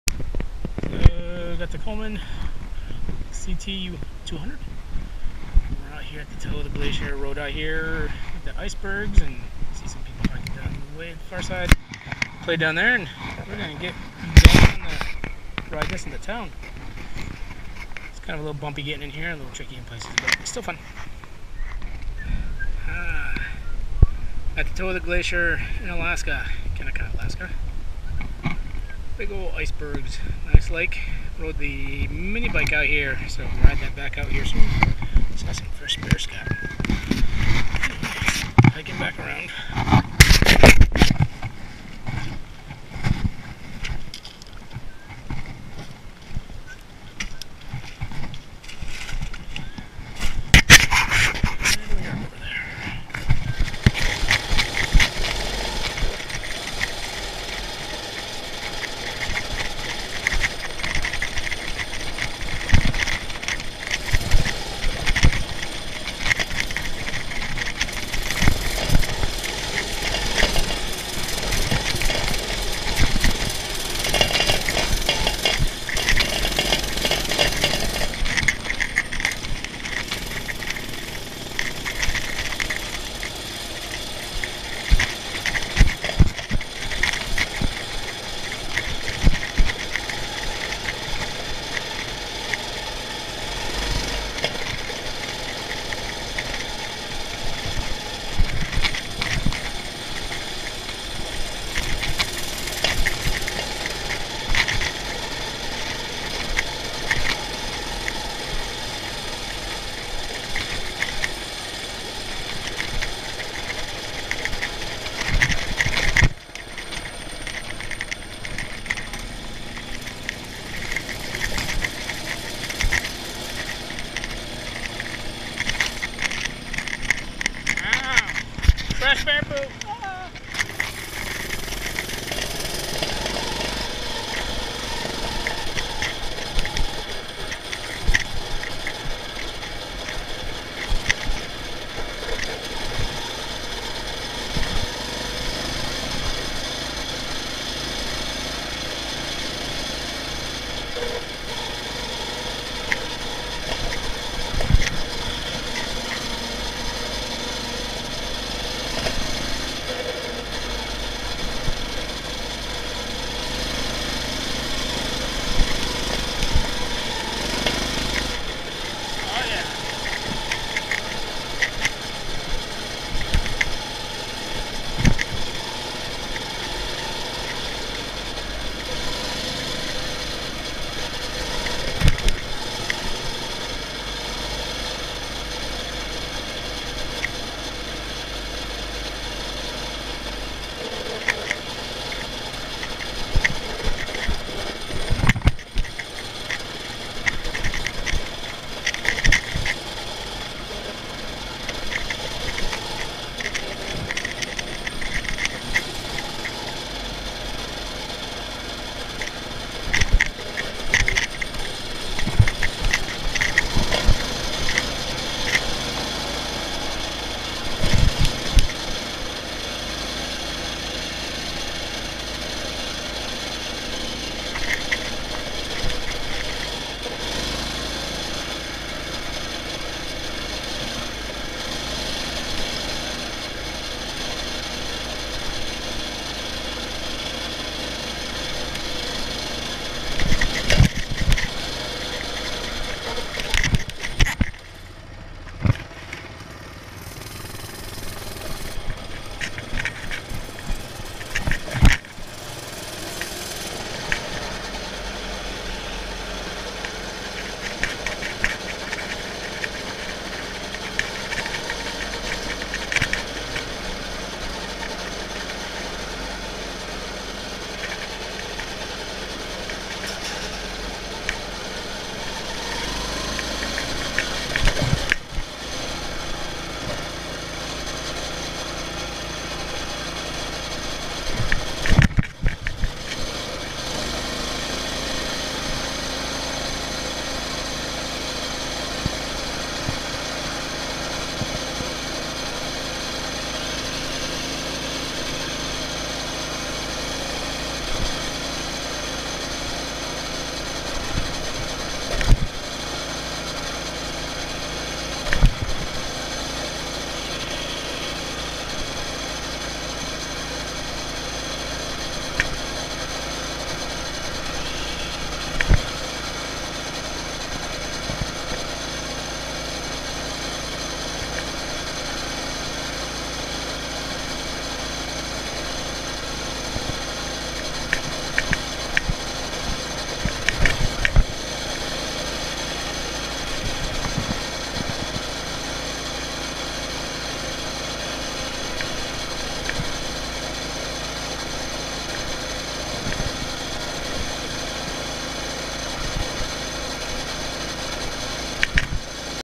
Coleman CT200: Chasing ice
I took the Coleman CT200 out to the Root Glacier in Mccarthy, Alaska. The bike is holding up well. This is my first ride on it since last summer and the clutch stuck once, but then worked ok. It is holding up well to the abuse I've given it out there. Fun ride! We saw fresh bear scat, but thankfully no bears.